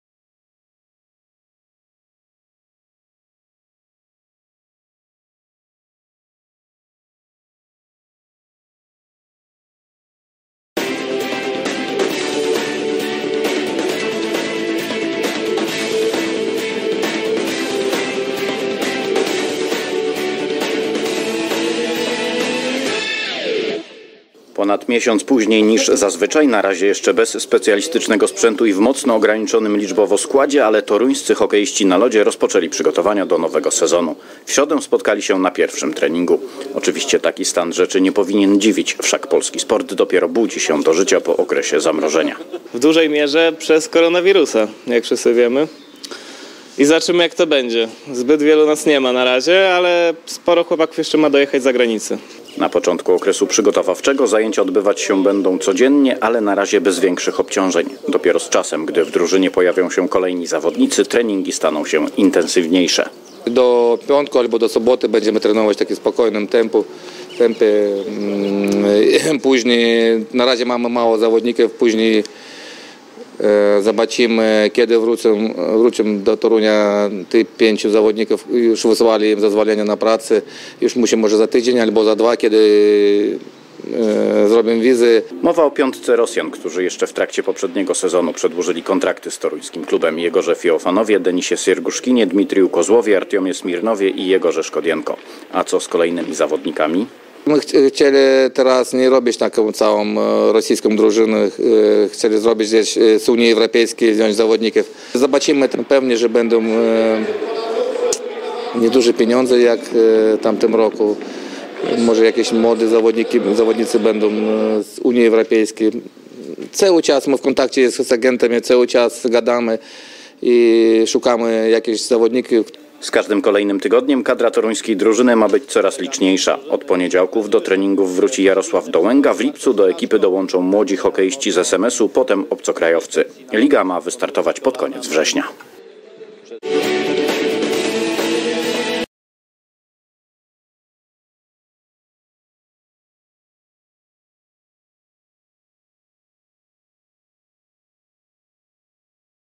Odmrażanie hokeja na lodzie
Na wczorajszym treningu gościliśmy Jakuba Pieczatowskiego z TV TORUŃ, który przyjrzał się temu jak "odmraża się" hokej na lodzie latem.